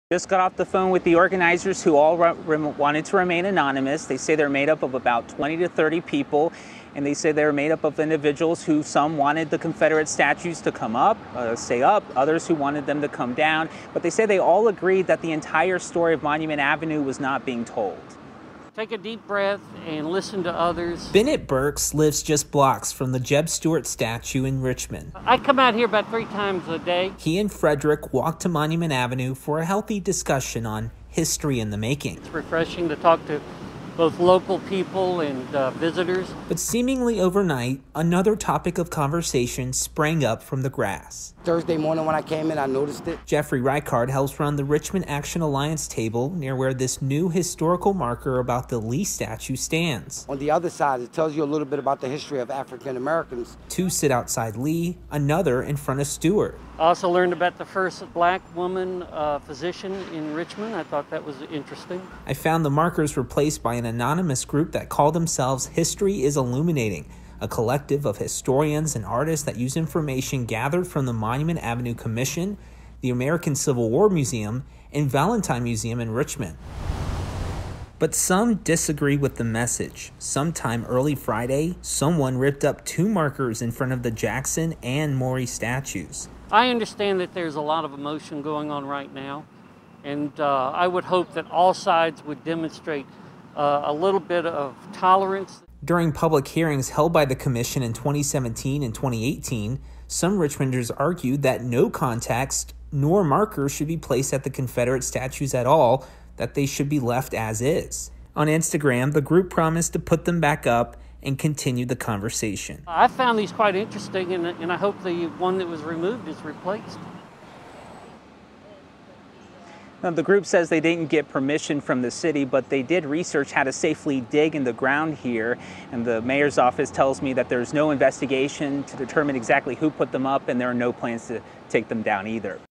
Group's historical markers on Monument Avenue taken down
A group of local historians and artists have planted giant historical markers in front of several Confederate statues along Richmond’s Monument Avenue.